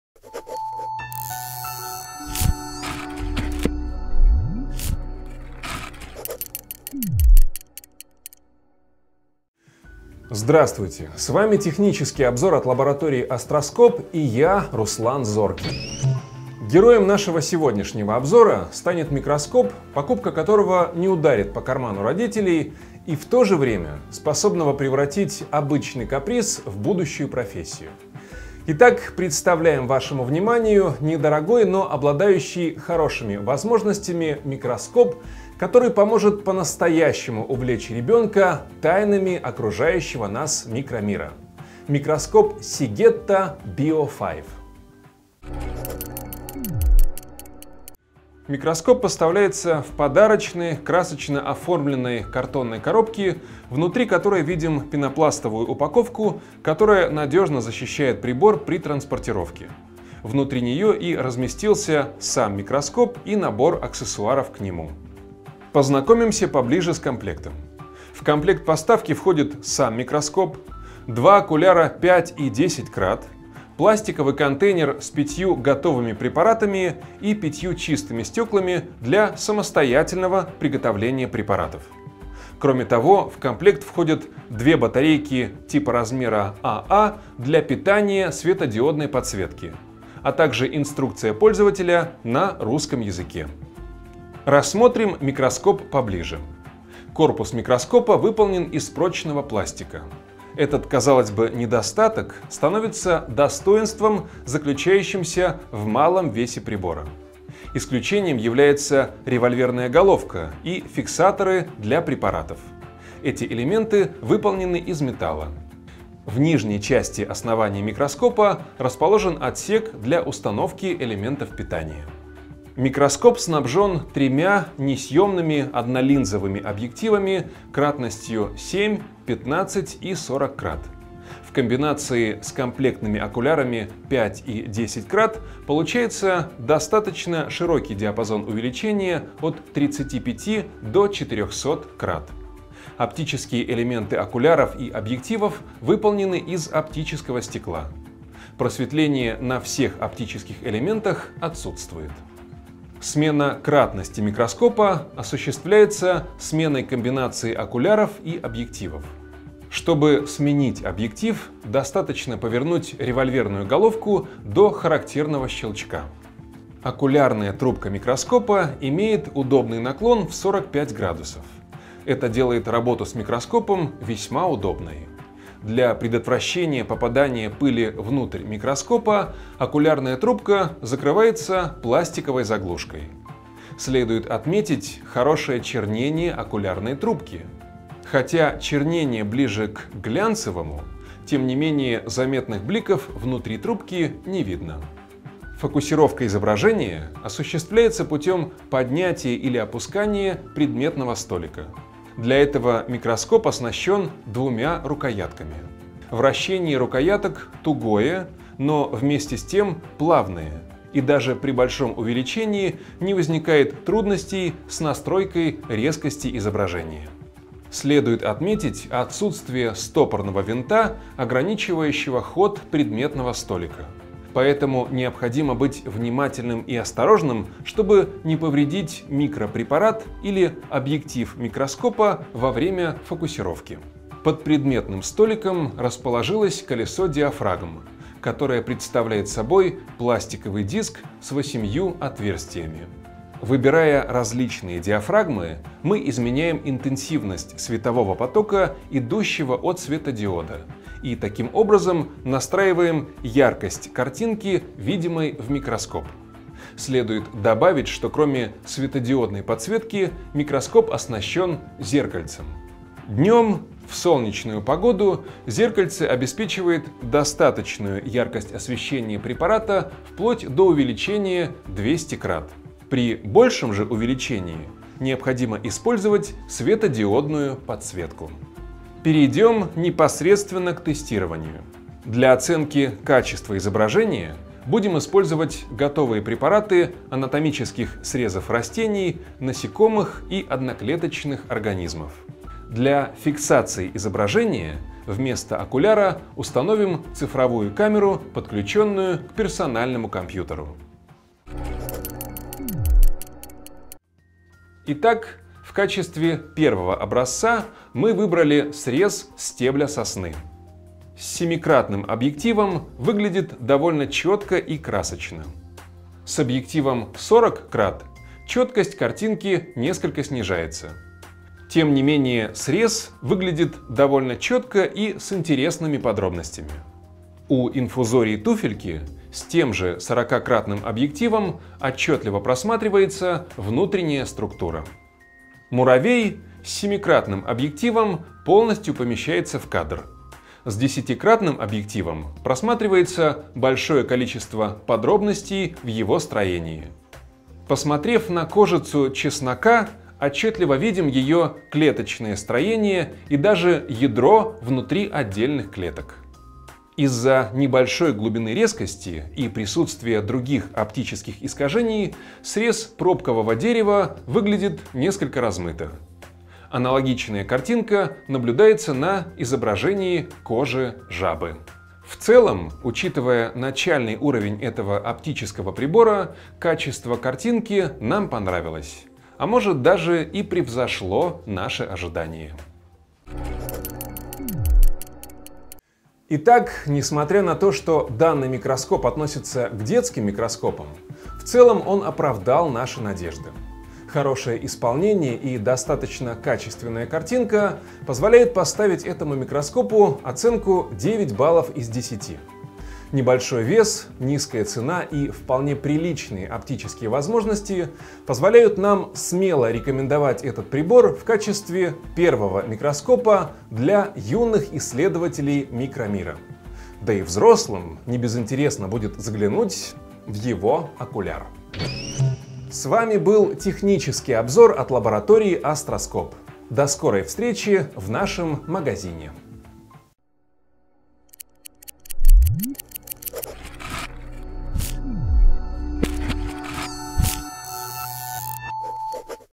Обзор микроскопа Sigeta BioFive
Sigeta Bio Five - отличный микроскоп начального уровня с качественной стеклянной оптикой.